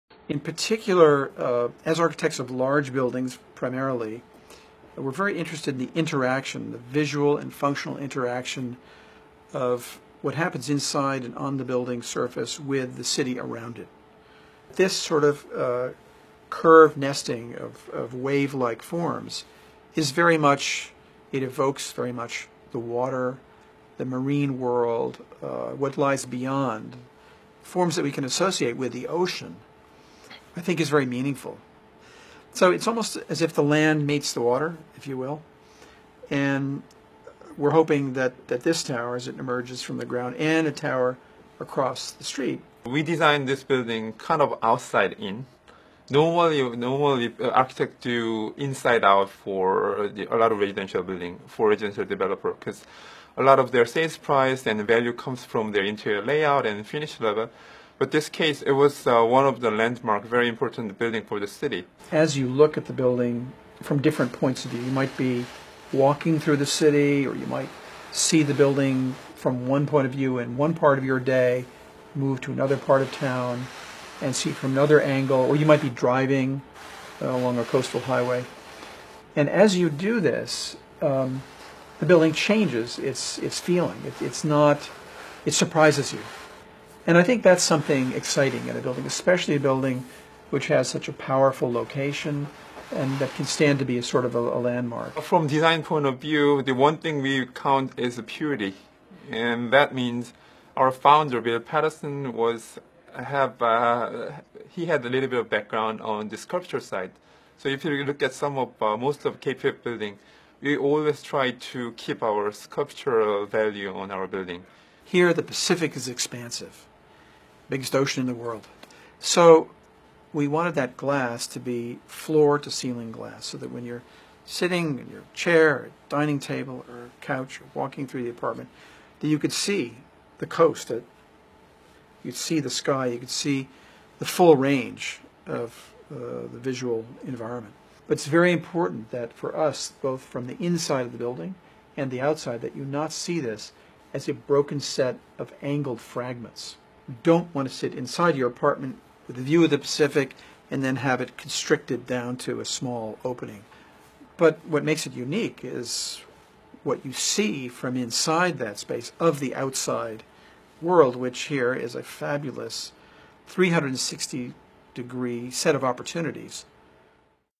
Jamie von Klemperer of KPF Discusses Pacific Gate San Diego by BOSA
Jamie von Klemperer president of the powerhouse NY based architectural design firm KPF discusses his vision and design inspiration for Pacific Gate San Diego.

Video Courtesy of: BOSA Development & KPF